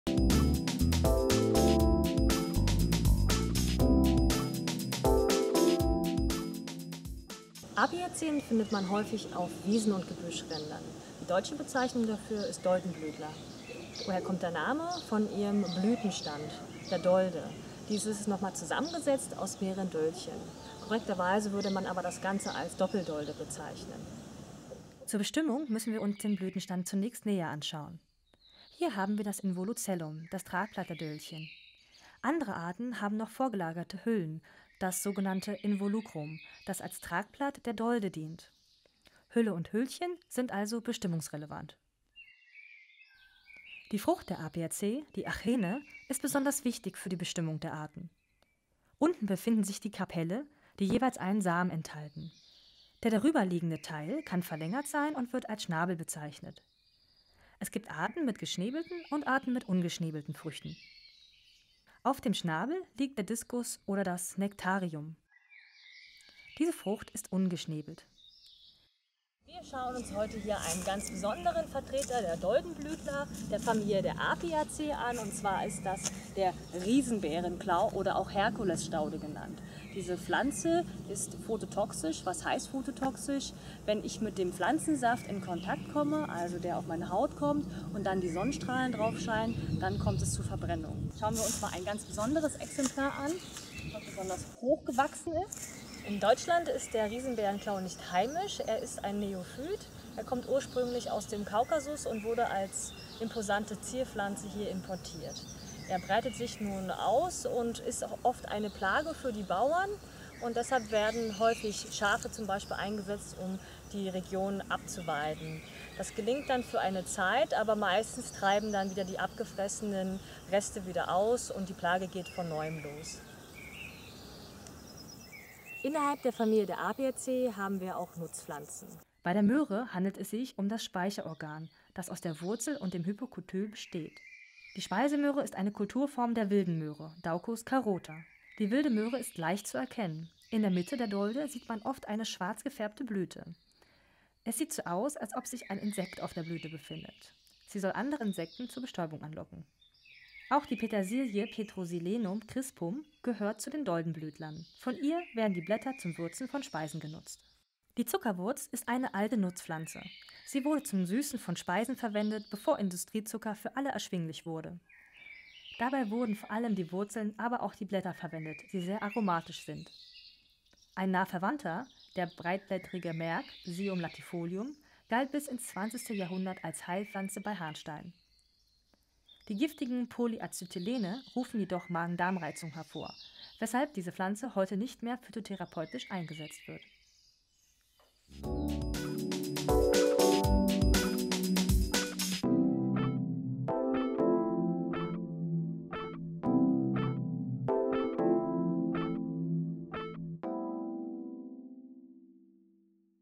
Apiaceae / Doldenblüter
Lerne die wichtigsten einheimischen Pflanzenfamilien zu unterscheiden. Unser Mini-Online-Bestimmungskurs hilft dabei.
Zur Vorbereitung auf die universitären Bestimmungskurse, zum Wiederholen für Fortgeschrittene oder auch für Botanil-Interessierte.